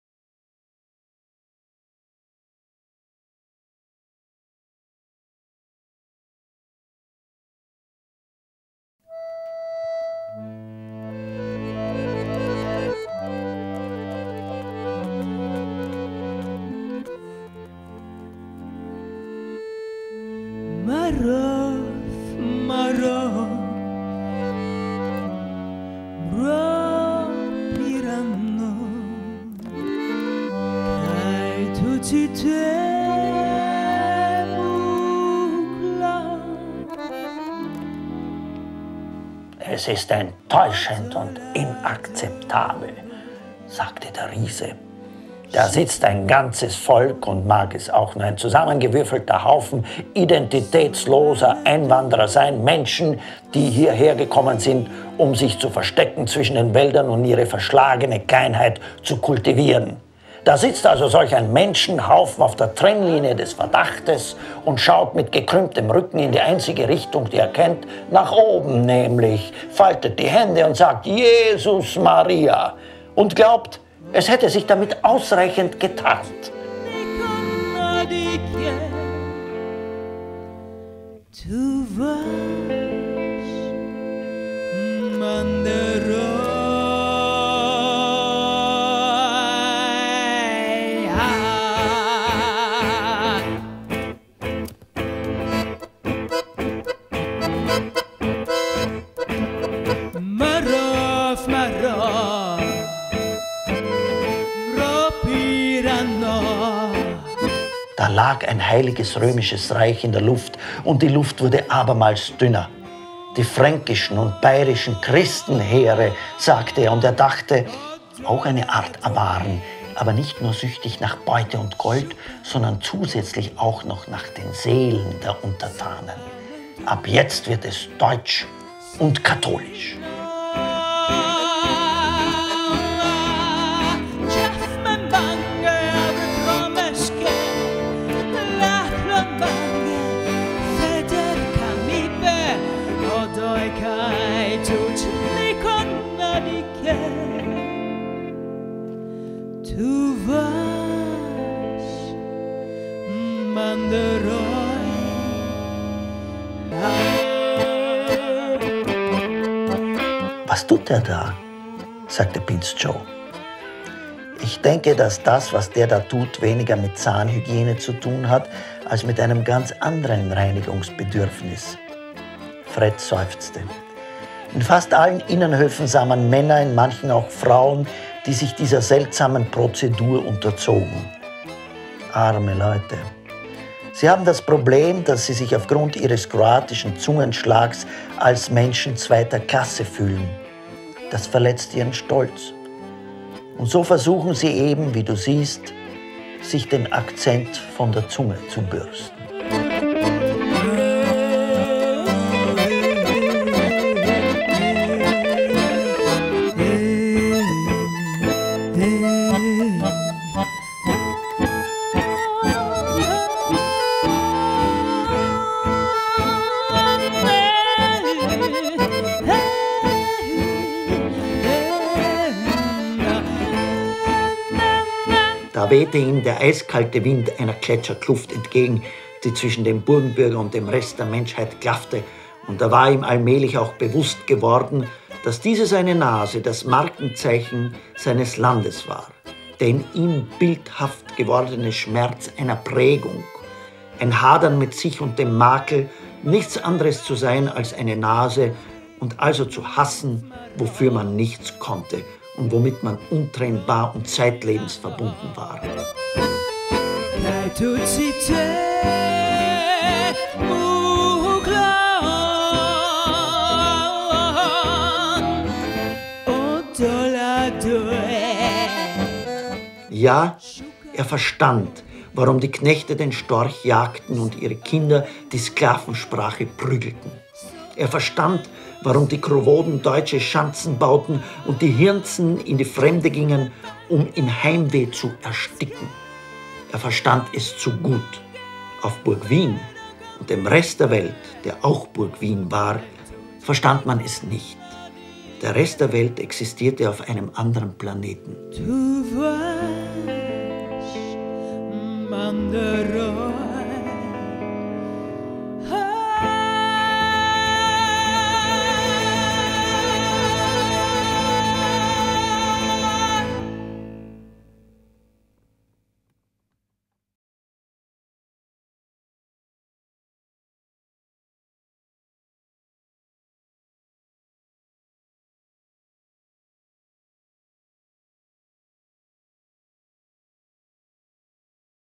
Burgenbürger_Romansatire von Peter Wagner_CleanTrailer.m4v
Tauchen Sie ein in Peter Wagners Romansatire DIE BURGENBÜRGER! Das Buch ist aber nicht nur 568 Seiten stark und reichhaltig illustriert, sondern auch Basis für eine Performance zwischen Lesung, Gesang und aberwitzig burlesken Geschichten und Gestalten.